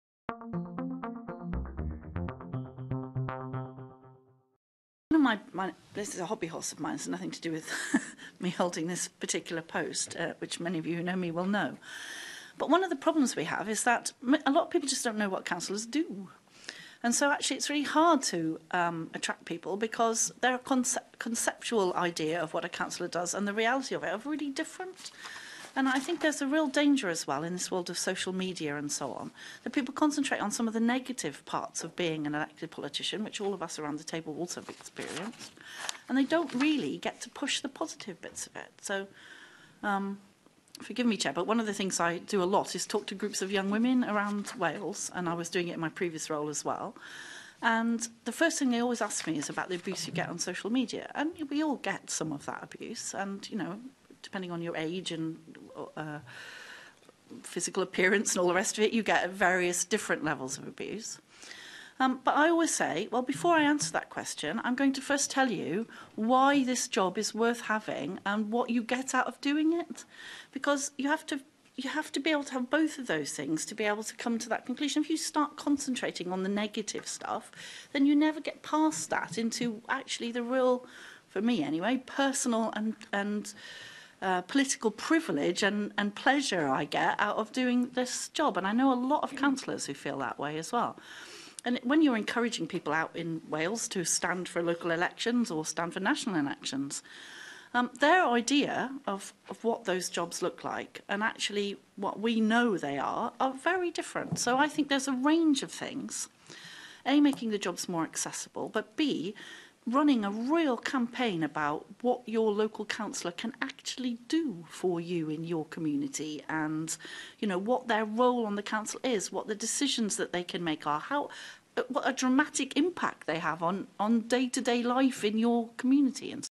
Positive aspects of being a politician should be emphasised
Local Government & Housing Minister, Julie James, tells the Senedd's Communities Committee that people may be put off  running for local government because they don't know what councillors do or have the wrong idea about political life.